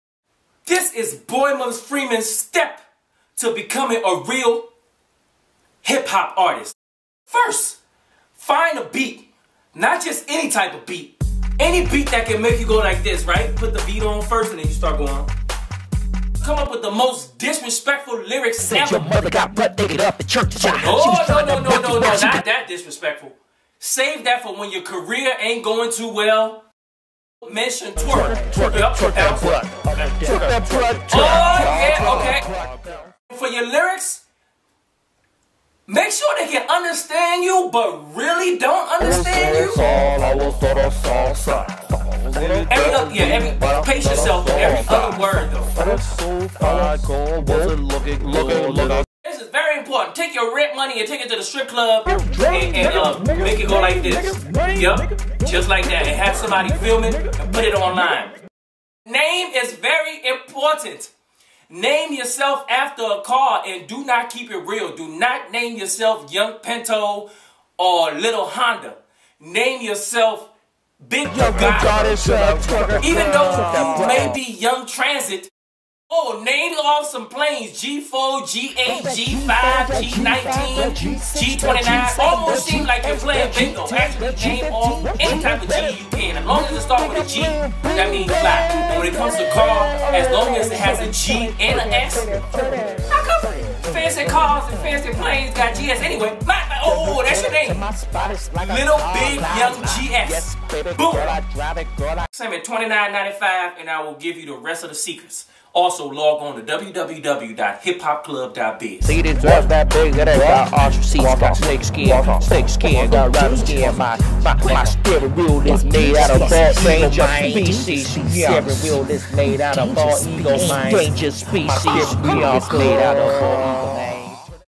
10 rules to being a rapper 2014
Boima Freeman shows you how you too can become a successful rapper in 2014 with little to no skills, just follow these 10 steps, of course some of the steps are missing, find out why at the end.